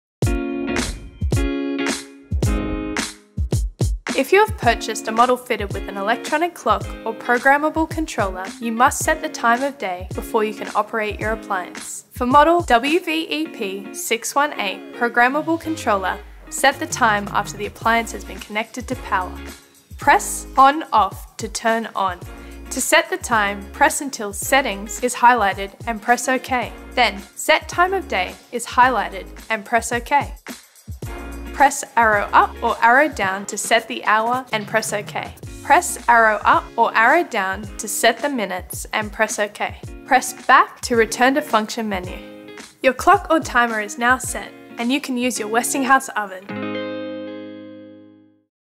Westinghouse Ovens - How to Set Your Clock Time (WVEP618)
If your Westinghouse oven is fitted with an electronic timer, you must set the time before you can use it.

For model WVEP618 programmable controller, set the time once the appliance is connected to power.

1. Press the ON/OFF button to turn on.
2. Press the arrow until Settings is highlighted and press OK.
3. Press the arrow until Set Time of Day is highlighted and press OK.
4. Set the time of day by pressing the up/down arrow for hour and minutes, and press OK to confirm.